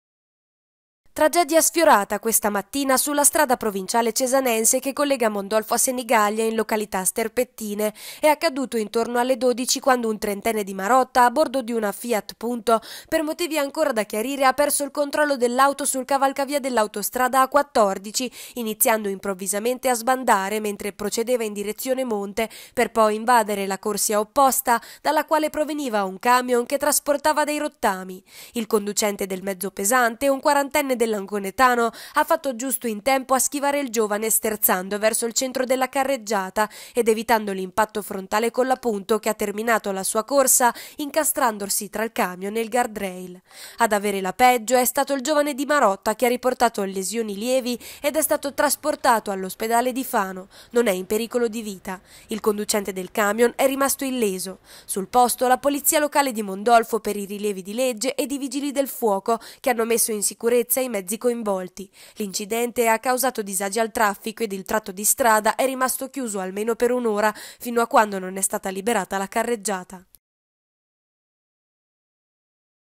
Sbanda e si incastra tra camion e guardrail
Ha cominciato a sbandare per la strada, per poi finire incastrato tra un camion che transitava sulla corsia opposta ed il guardrail. Momenti di paura sulla Strada Provinciale Cesanense, dove un giovane di Marotta, ha perso il controllo della sua auto.